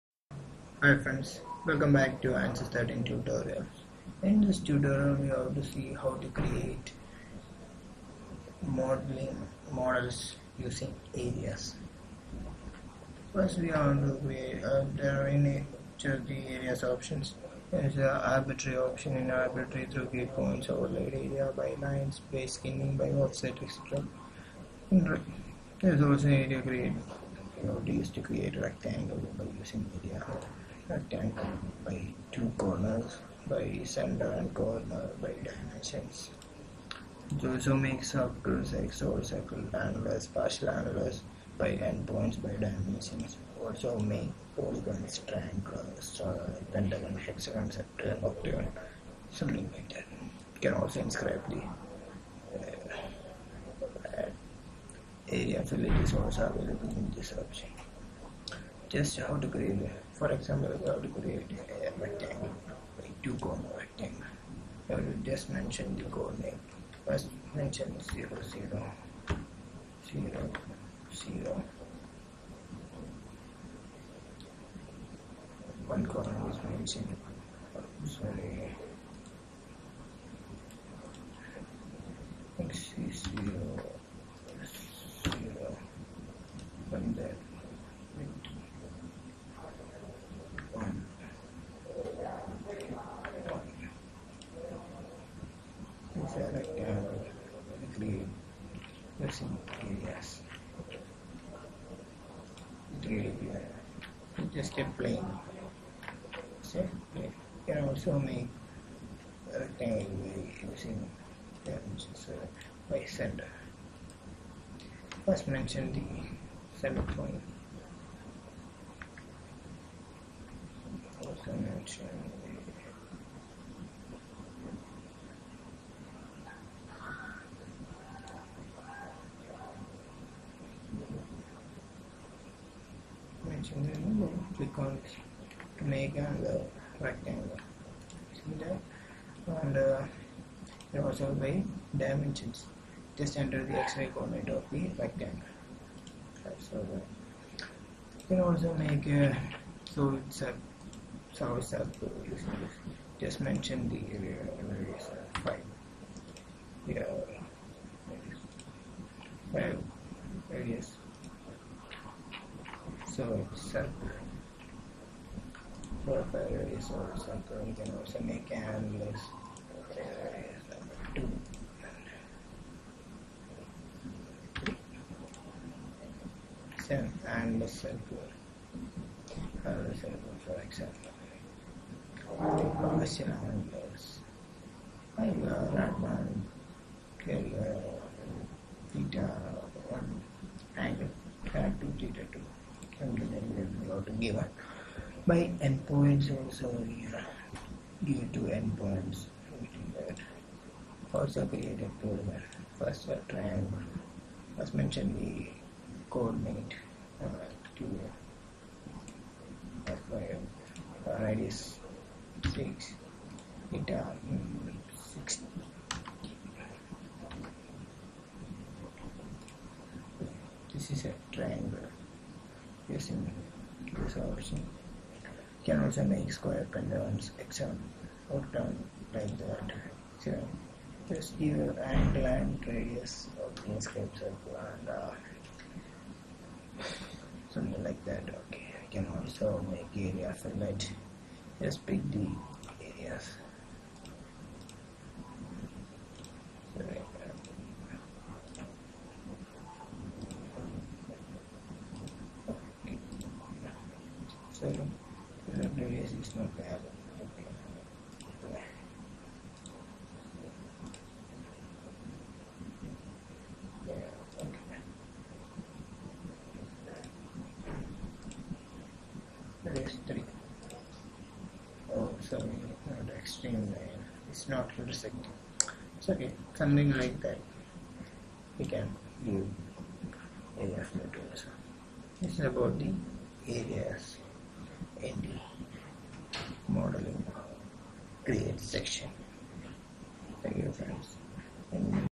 Ansys 13 Tutorials Free Download_How to Use Create option in Modelling for Areas in Mechanical APDL
This video is about Ansys 13 Tutorials Free Download_How to Use Create option in Modelling Section for Areas in Mechanical APDL.